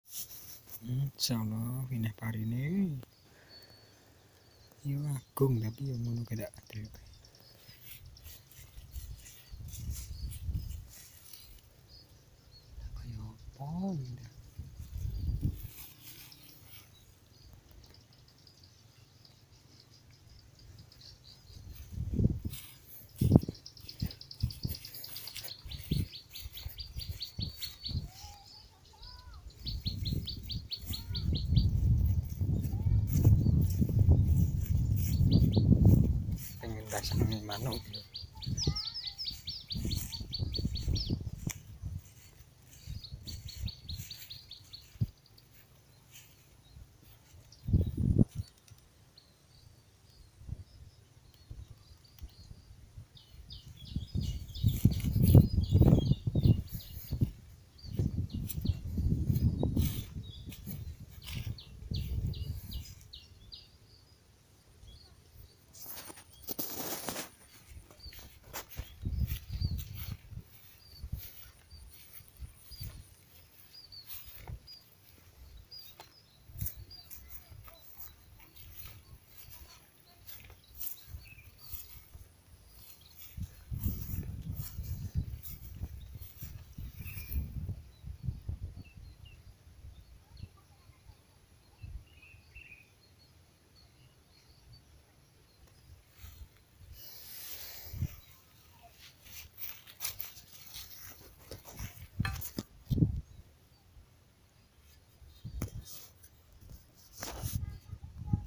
Kondisi Sawah Setelah Panen Padi | Kampung Bojonegoro
Hallo gesss

Di video kali ini saya berbagi video tentang suasana sawah setelah panen padi

semoga terhibur terima kasih